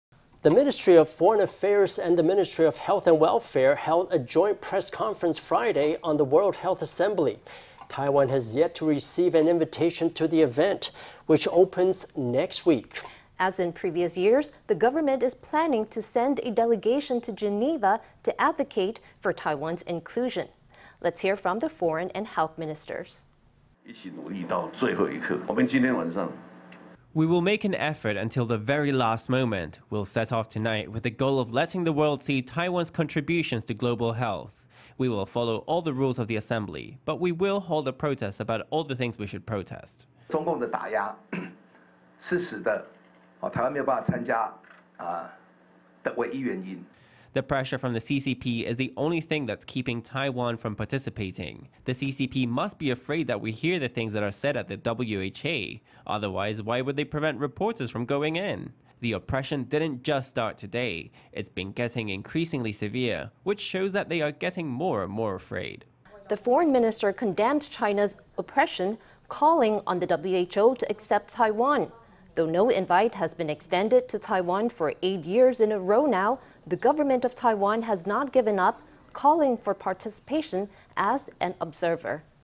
Foreign and health ministers to advocate for Taiwan’s WHA inclusion in Geneva｜Taiwan News
The Ministry of Foreign Affairs and the Ministry of Health and Welfare held a joint press conference Friday on the World Health Assembly. Taiwan has yet to receive an invitation to the event, which opens next week. As in previous years, the government is planning to send a delegation to Geneva to advocate for Taiwan’s inclusion. Let’s hear from the foreign and health ministers.

Chiu Tai-yuan
Health minister
We will make an effort until the very last moment. We’ll set off tonight with the goal of letting the world see Taiwan’s contributions to global health. We will follow all the rules of the assembly. But we will hold a protest about all the things we should protest.

Lin Chia-lung
Foreign minister
The pressure from the CCP is the only thing that’s keeping Taiwan from participating. The CCP must be afraid that we hear the things that are said at the WHA. Otherwise, why would they prevent reporters from going in? The oppression didn’t just start today. It’s been getting increasingly severe, which shows that they are getting more and more afraid.

The foreign minister condemned China’s oppression, calling on the WHO to accept Taiwan. Though no invite has been extended to Taiwan for eight years in a row now, the government of Taiwan has not given up, calling for participation as an observer.